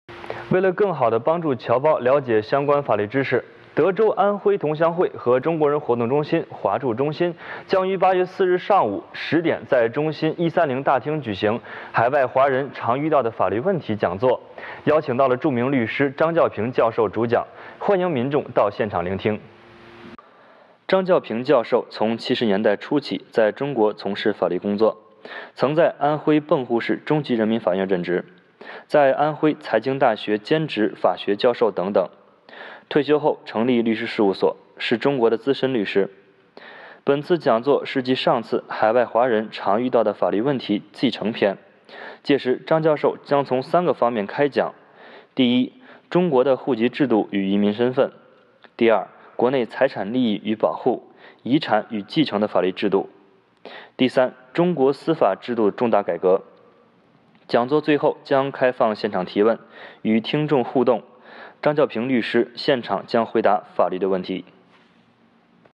「海外華人華僑常遇到的中國法律問題」講座 8月4日中國人活動中心舉行
為了更好的幫助僑胞了解相關法律知識，德州安徽同鄉會和中國人活動中心-華助中心將於8月4日周六上午10點在中心130大廳舉行「海外華人常遇到的法律問題」講座。邀請著名律師張教平教授主講，歡迎民眾到現場聆聽。